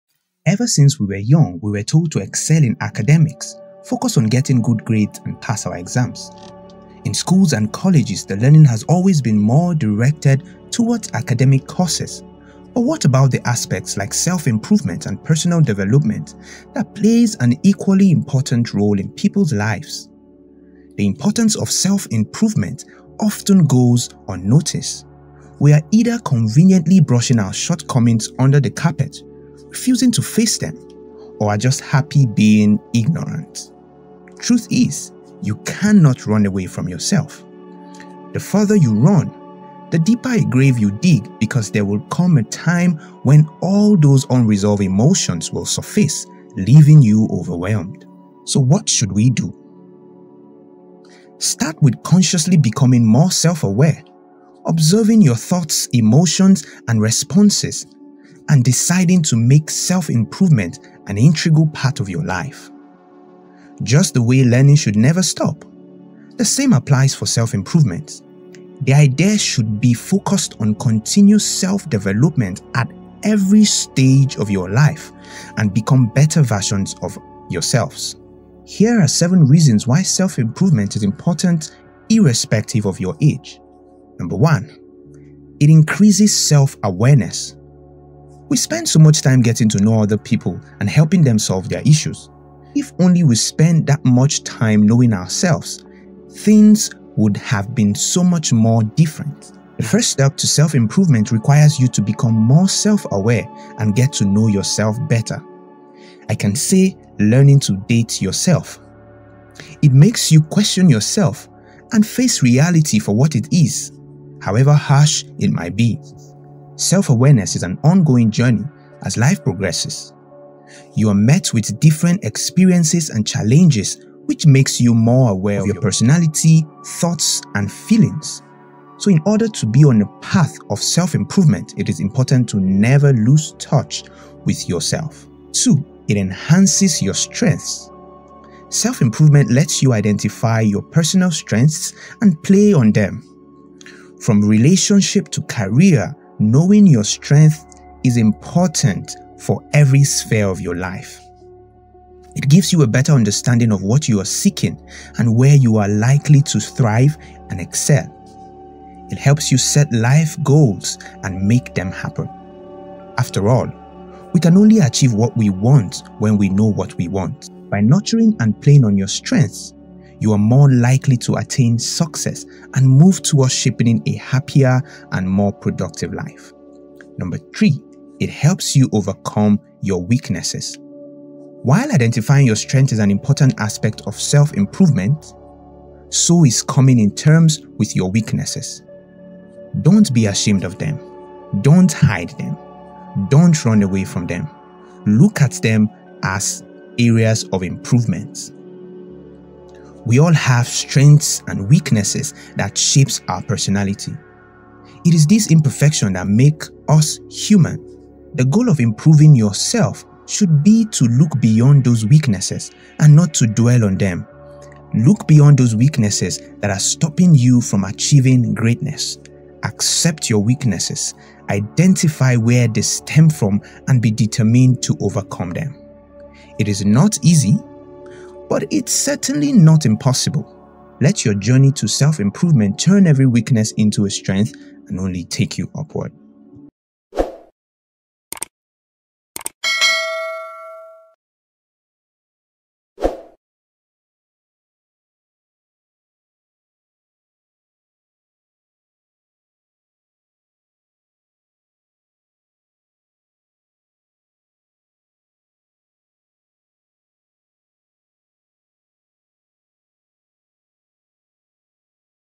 The importance of self improvement (no matter how old your are). Why you must self improve everyday.
Self Improvement, Personal Development & Growth - Ever since people began to realize the true meaning of happiness, peace, satisfaction, and success, they have constantly been in pursuit of it.

The importance of self improvement cannot be overlooked no matter how old you are.

Despite countless disasters, people have crafted ingenious ways to constantly improve the quality of their lives.

In this video, I'll be giving you 7 reasons why self improvement is important no matter how old you are.

Start taking charge of your life and wellbeing